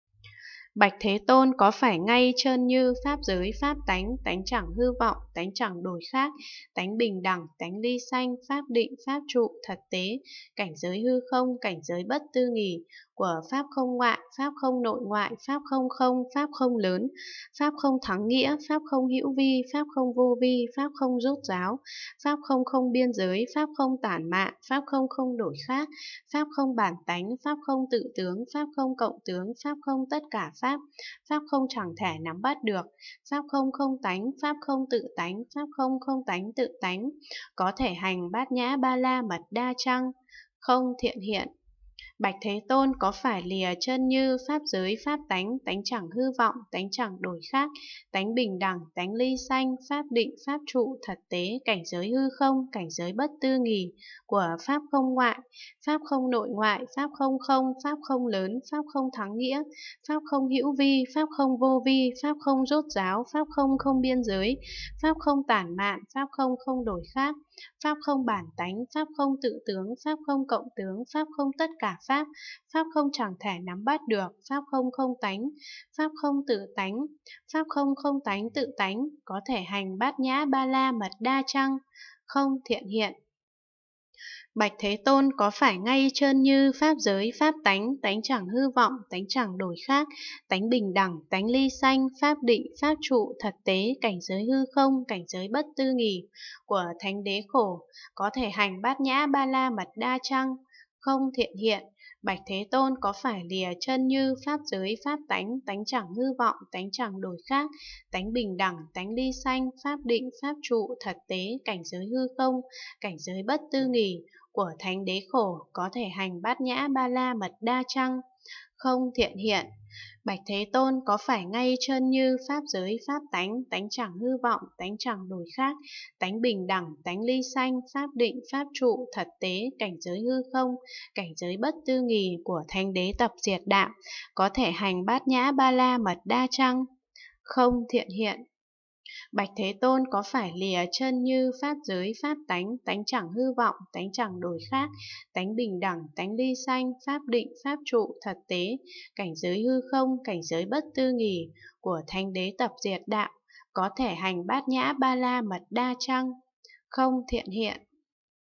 Quyển 336: Kinh đại bát nhã ba la mật đa - Phẩm đoạn phân biệt (rất hay) (336/600) (8)
Phẩm 54: Phẩm đoạn phân biệt
Hán dịch: Pháp sư Huyền Trang
Việt dịch: Hoà thượng Trí Nghiêm
Giảo chính: Hoà thượng Quảng Độ